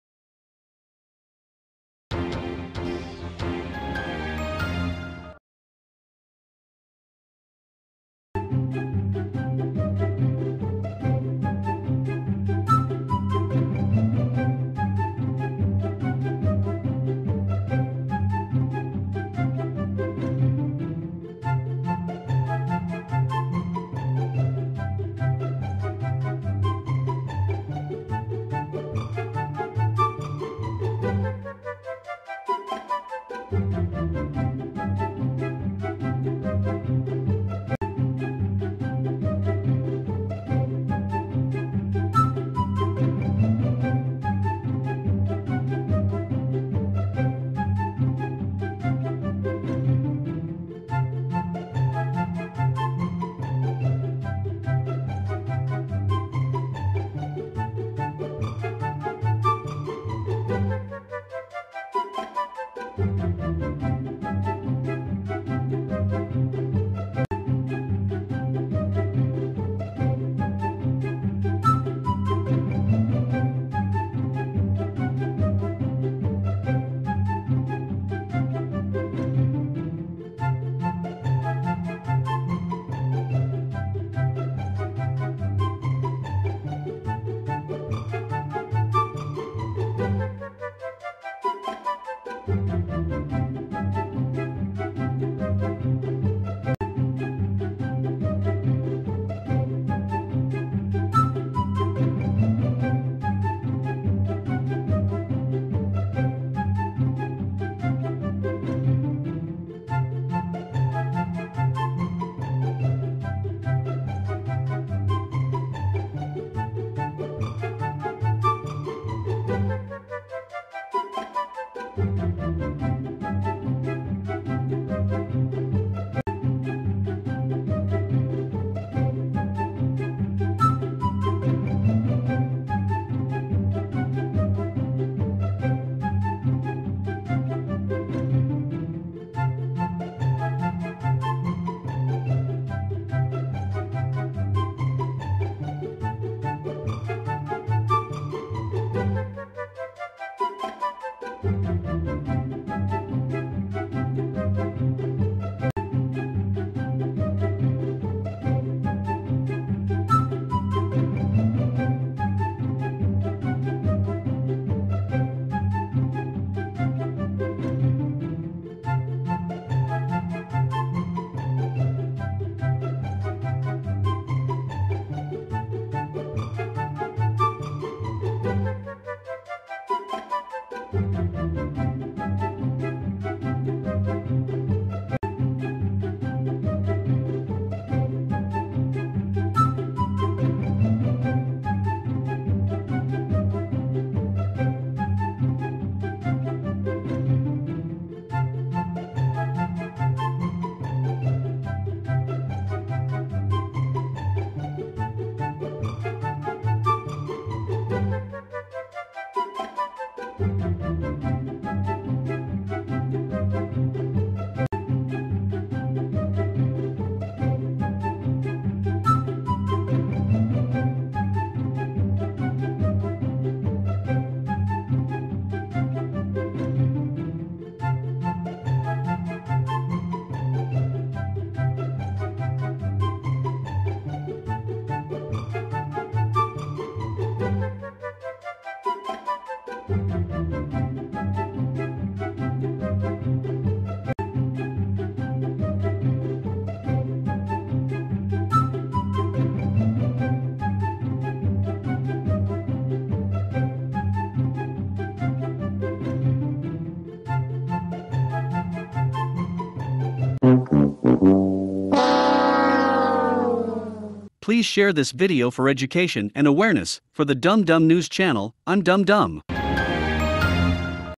CNN Fires Brian Stelter and Ratings Go Up and More Crazy News from Around the World...
CNN Fired Brian Stelter and Ratings Go Up and More Crazy News From Around The World...

Dum Dum